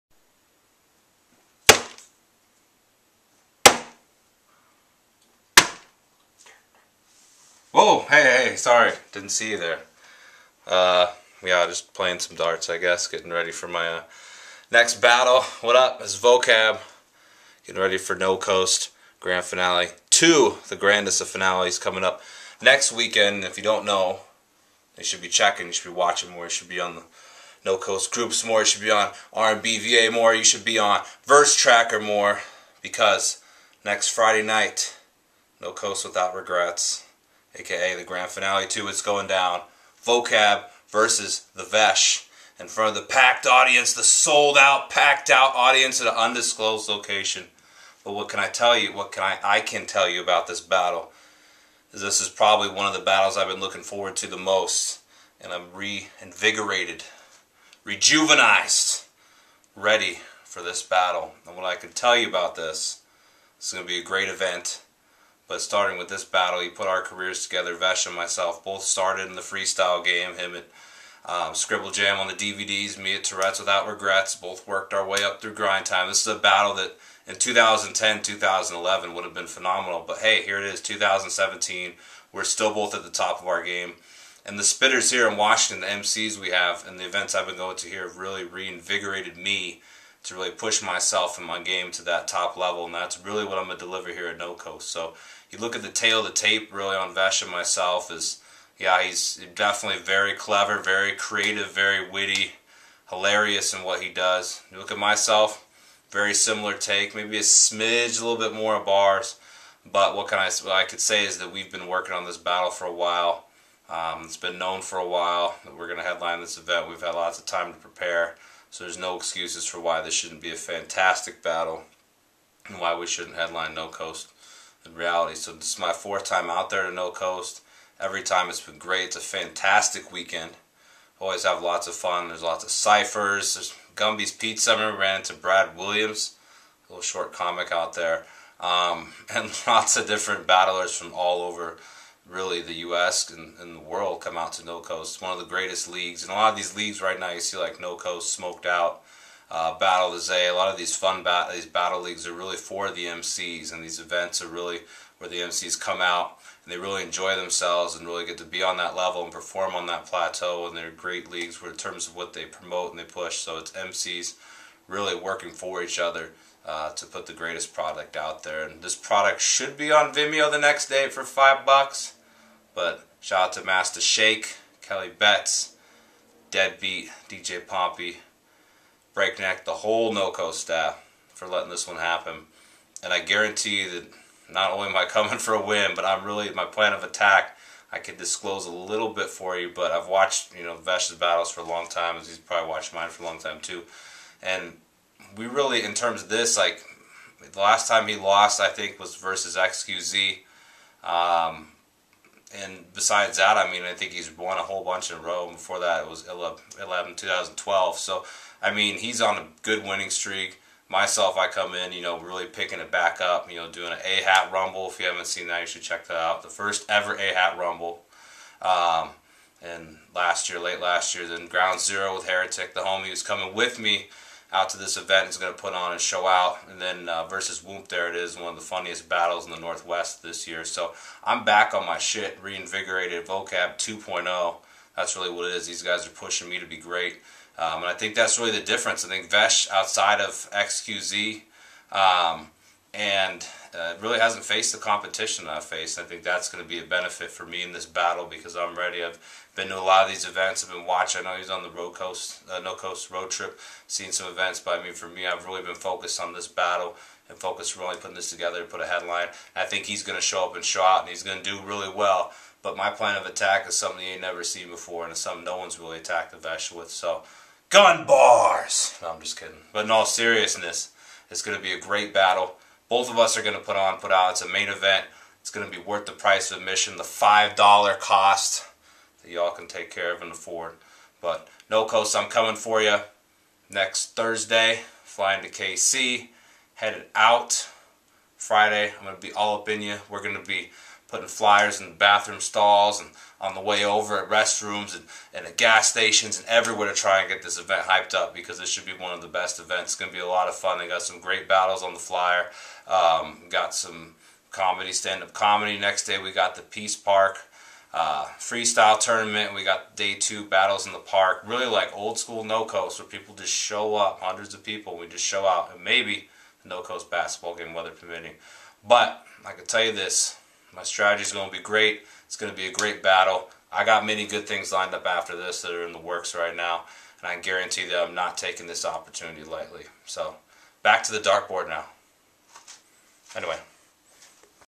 Vokab Vs Vesh No Coast Grand Finale 2 Preview Vid
Vokab breaks down his upcoming No Coast battle vs The Vesh @ No Coast Grand Finale 2 on April 28th.